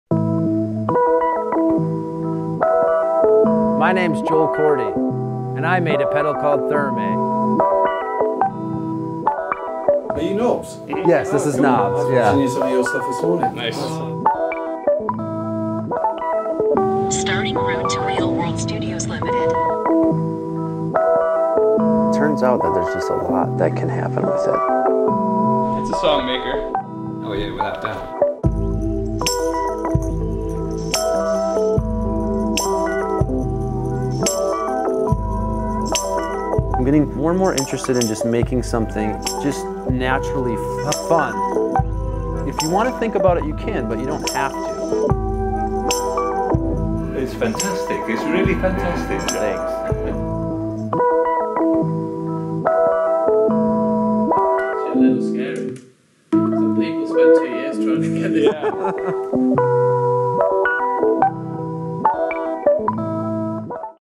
Thermae: Analog Delay / Pitch Shifter || Mini-Doc Trailer
I started this company to honor my lost brother and finally take his advice to “Follow Your Bliss.”  Five years on I’m still chasing it, and making Thermae gave me moments where I felt I caught pace.

This is a trailer to a larger mini-doc project that we will release next week.

Thermae pre-order starts today and can be ordered Thermae ships 5/31/18.

Much more info, specs, details to come as we approach the release date